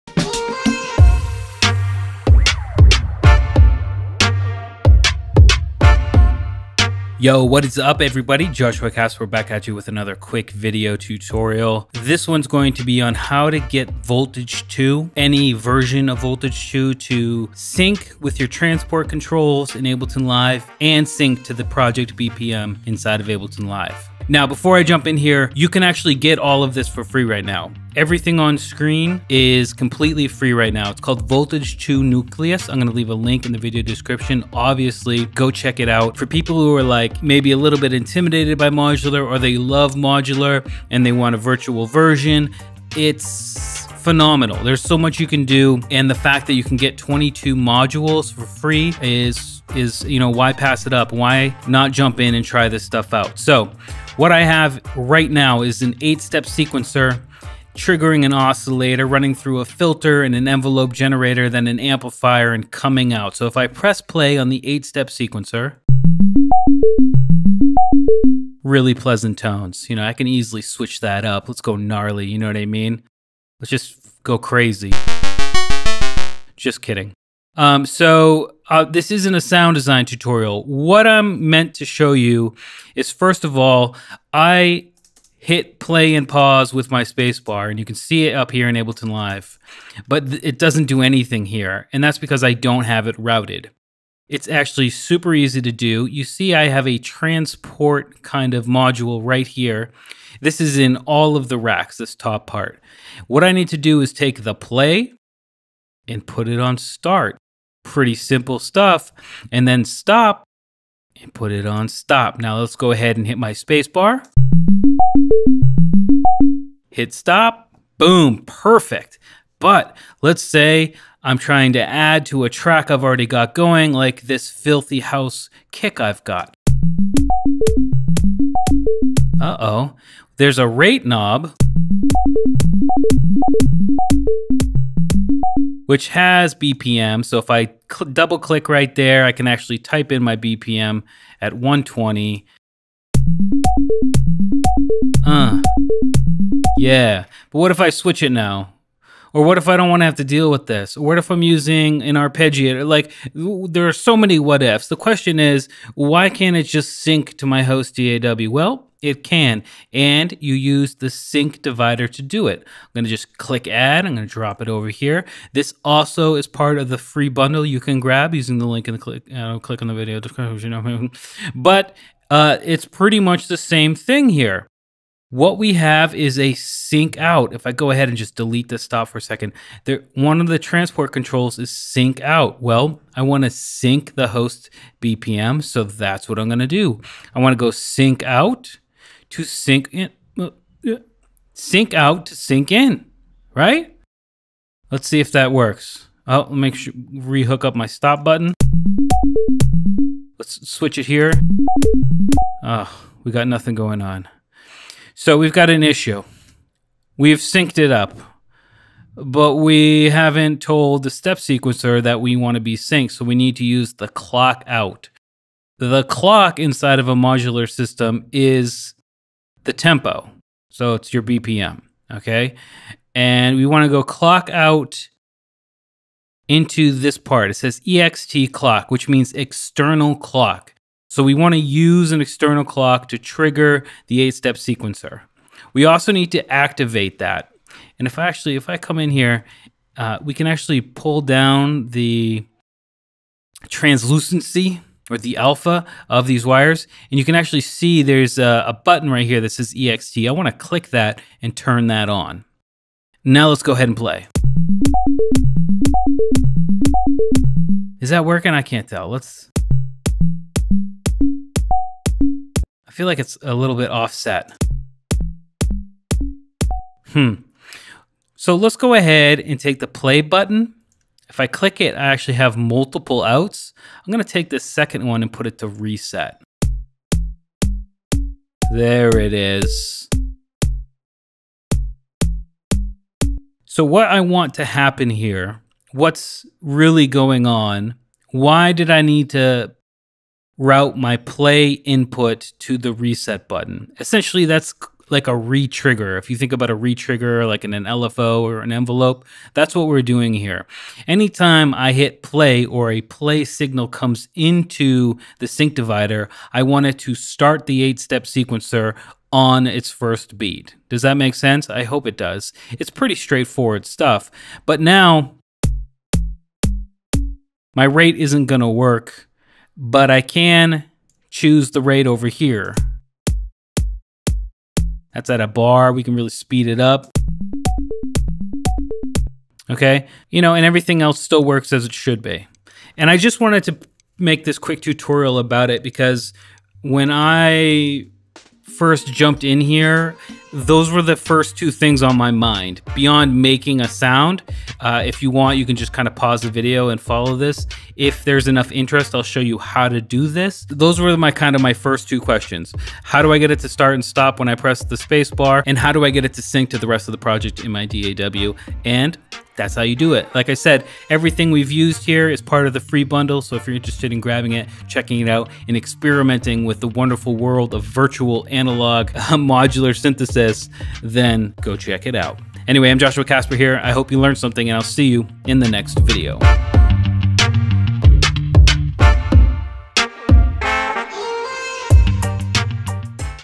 FREE | Get Voltage Modular Nucleus 2 & Tutorial on DAW BPM / Clock & Transport Sync
You can get 22 free Voltage Modular modules for free right now by downloading Voltage Modular Nucleus 2 today!!

There has never been a better time to see if you like patching in virtual modular system. The good people at Cherry Audio are giving away Voltage Modular Nucleus 2. It is the base VTS Plugin system and 22 modules to patch with in any DAW.

Tutorial – Getting Started with Host Sync Transport Controls & BPM

When I initially jumped into Voltage Modular, the first couple of questions I had were how to start and stop its sequencers with my space bar, just like in my DAW, and how to get the sequencers and arpeggiators to sync with Ableton Live’s BPM. Luckily, it wasn’t too difficult to figure out.

I figured it would be a cool thing to go ahead and make a video tutorial on the process, explaining as much as I could along the way to help anyone else looking to jump into virtual modular shenanigans.

Syncing Voltage Modular with the DAW Transport Controls
This one is super easy and straight forward.

Any sequencer in Voltage 2, including the free 8 Step Sequencer in Nucleus, will have a start and stop button and a corresponding patch slot available.

In the I/O bar at the top of the GUI, which is automatically loaded in every instance of the Voltage plugin, there is a section called “Transport”. There you will find 4 patch options; Play, Stop, Sync Out, Play Gate.

We want to connect the Play here with the Play on the Sequencer and the same for the 2 Stops.

That’s it. Now, when you smash that space bar, or hit play in your DAW the 8 Step Sequencer should start too and when you stop everything the 8 Step Sequencer will stop too.

Syncing Voltage Modular with the DAW BPM
To sync Voltage Modular’s sequencers, LFOs, Arpeggios and everything else with the DAW’s BPM we will need to add the Sync Divider Module. Don’t worry, this module is also a part of the Free Voltage Modular Nucleus 2 download.

There are 3 patching areas on the module and we need to fill them up with the appropriate routing.

The Sync In on the Sync Divider goes to the Sync Out in the Transport controls from the last part of the tutorial. This is feeding the BPM / Tempo / Clock information into the module.

In modular speak, Clock is BPM or Tempo. Therefore, we need to patch the “Clock Out” at the bottom of the Sync Divider module to the EXT CLK (External Clock) on the 8 Step Sequencer. We also need to turn on this function by hitting the EXT button.

Now, we have successfully fed the DAW’s BPM into Voltage Modular and we can use the Sync Dividers tempo options to choose how to divide up that BPM.

Now, there is one more thing to do. Right now, as we start and stop the 8 Step Sequencer, the last known position is where it will start each time. We want it to reset and start again every time we hit play. To do that, we simply need to route the Play output from the Transport controls into the Reset input on the Sync Divider.

This acts as a retrigger. Every time a play signal comes in, not only does it start the 8 Step Sequencer, it also resets it to make it play from the beginning.